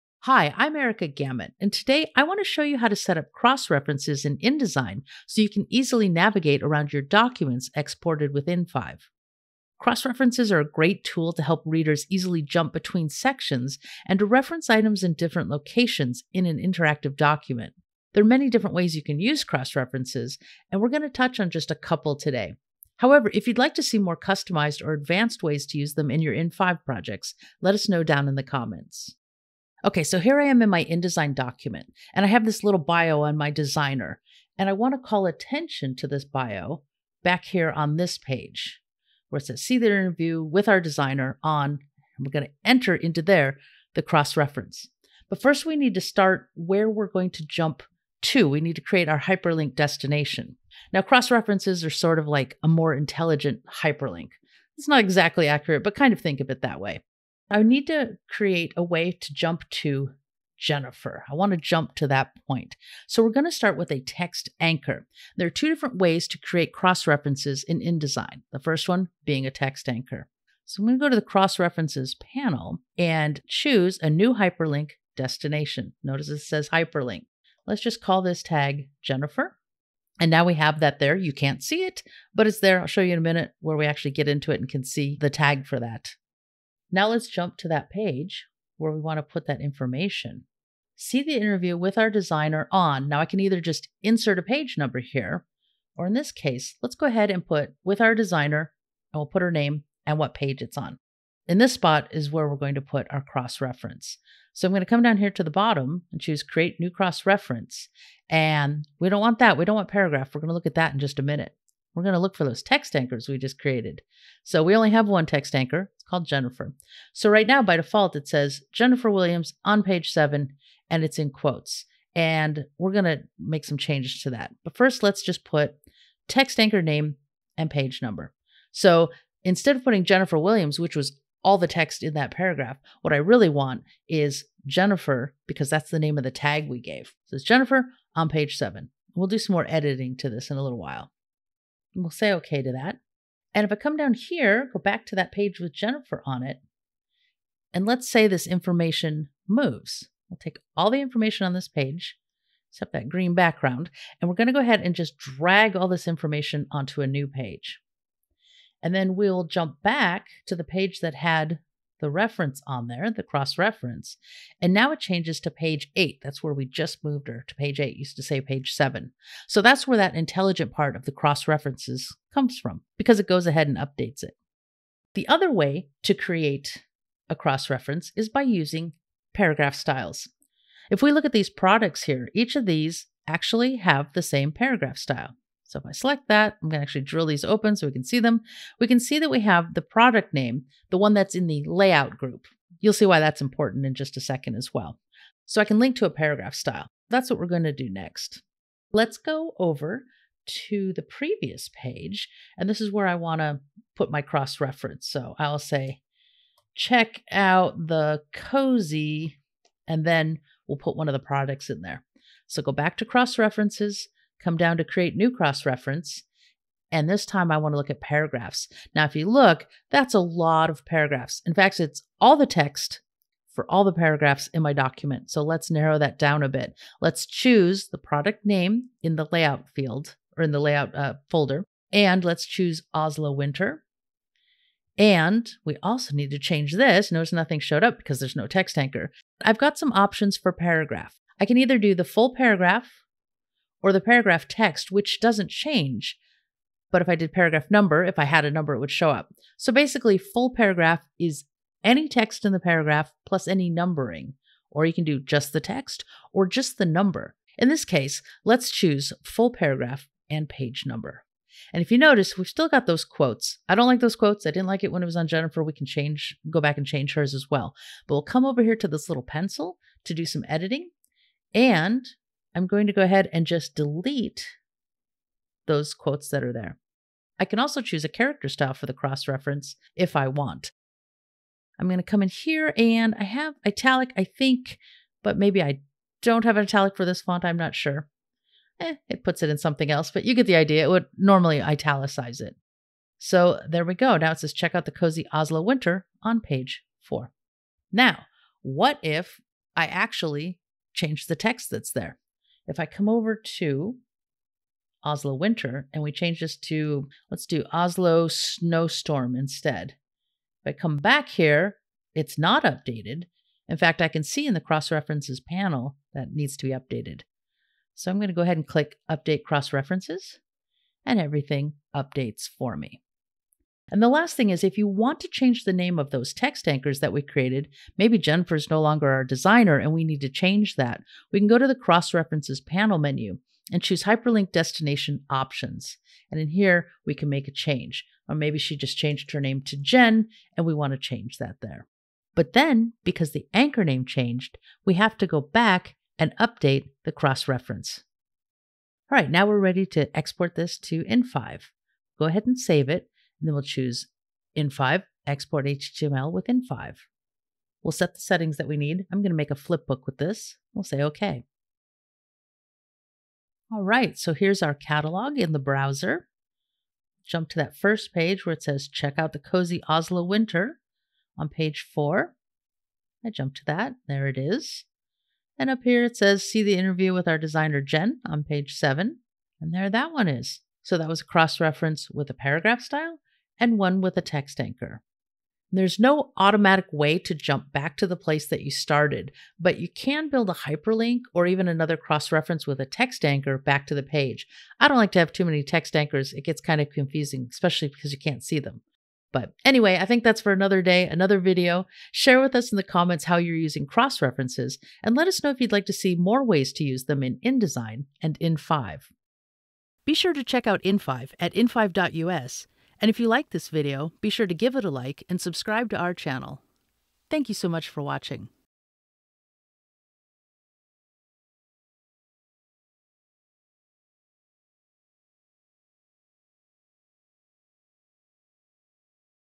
Cross References in Digital Publications with InDesign & in5
Learn how to use xrefs to create dynamic links between pages that can hold custom text.